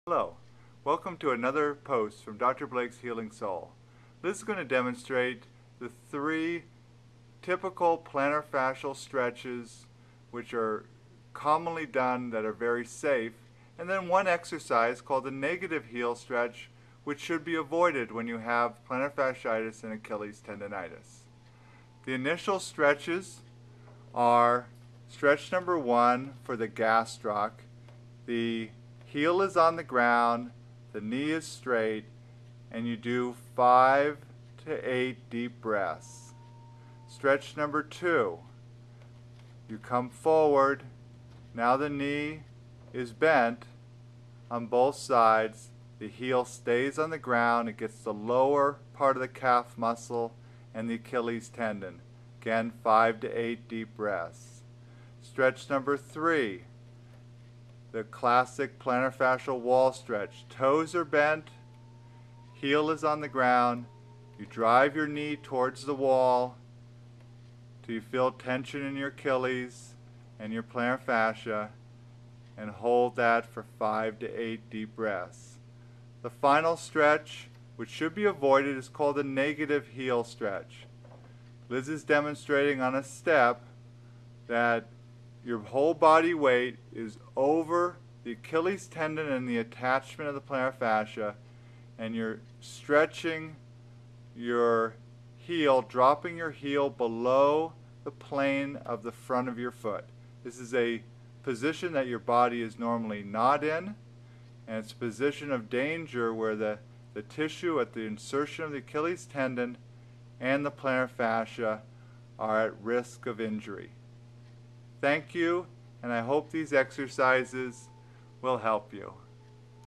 How To Stretch for Plantar Fasciitis and Achilles Tendinitis
4 Common Stretches for Plantar Fasciitis and Achilles Tendinitis are demonstrated. The Negative Heel Stretch (the final one) should be avoided when these injuries occur. The Gastroc Stretch, The Soleus Stretch and the Plantar Fascial Wall Stretches are all safe to do with plantar fasciitis and achilles tendinitis. Never stretch with pain. See Dr Blake's blog for more insight into these problems